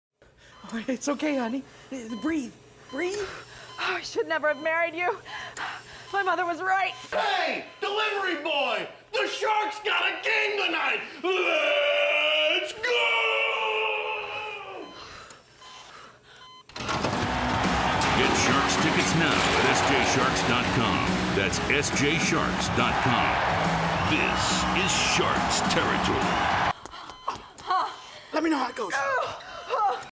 Delivery Boy 1/30/2013
The latest Sharks hockey commercial: Delivery Boy. San Jose Sharks tickets are on sale now at SJSHARKS.com.